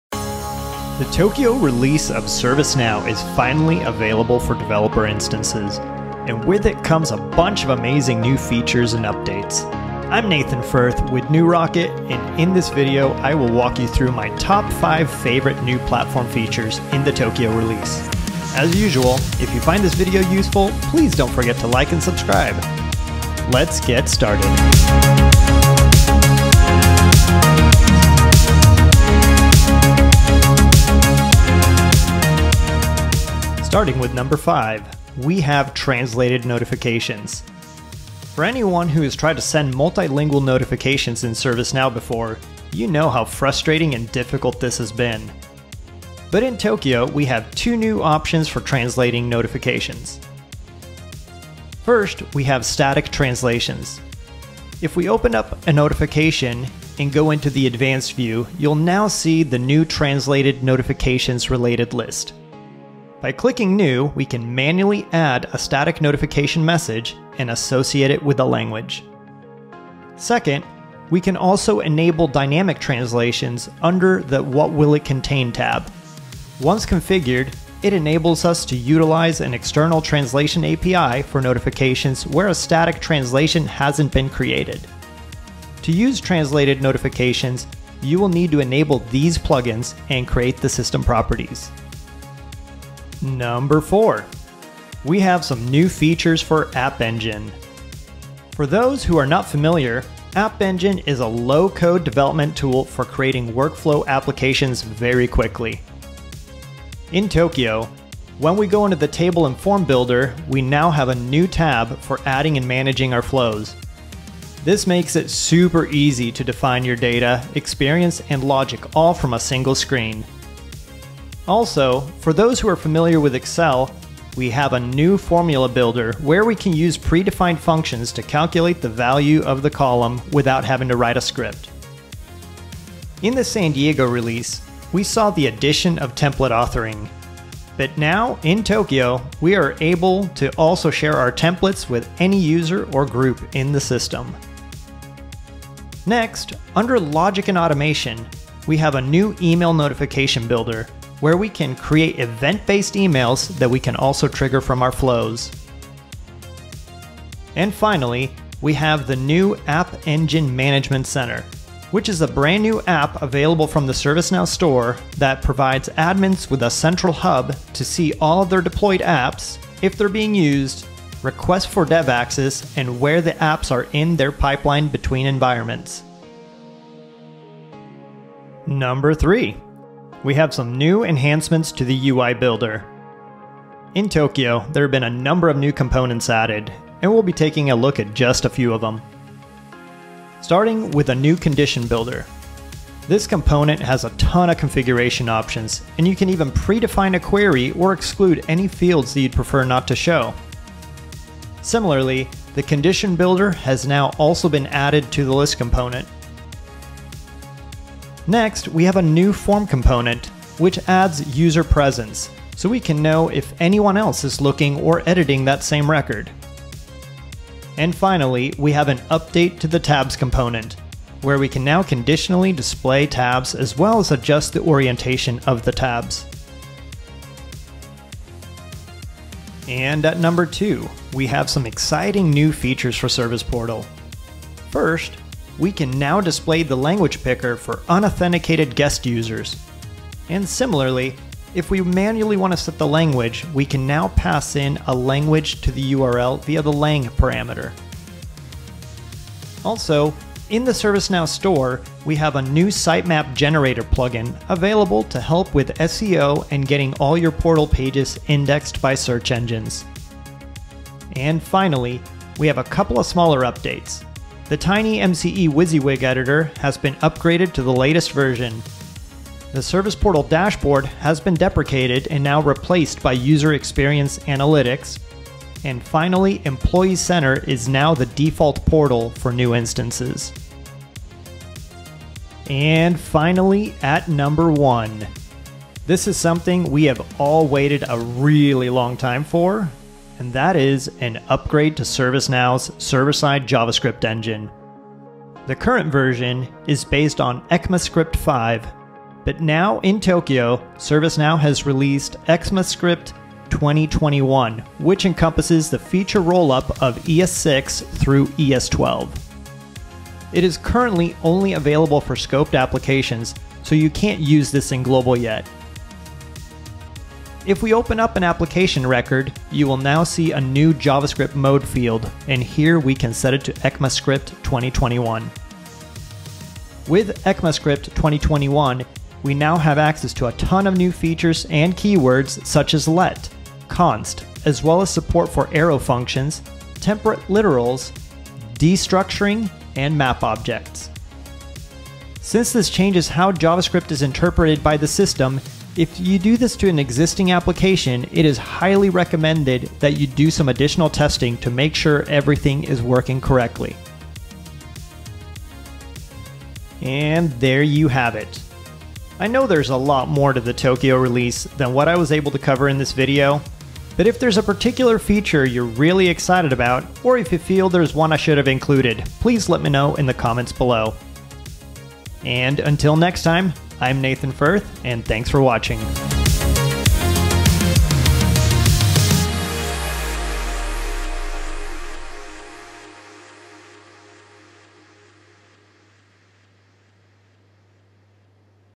Top 5 New Features in the ServiceNow Tokyo Release!
The Tokyo release of ServiceNow is available for developer instances and comes with many new features and updates. In this video, I will walk you through my favorite new platform features and give you a closer look at the Tokyo release.

Including updates for:
- 00:00 Overview
- 0:34 Translated Notifications
- 1:34 App Engine
- 2:58 UI Builder
- 3:50 Service Portal
- 4:42 ECMAScript 2021
- 5:55 Top 5 Features in Tokyo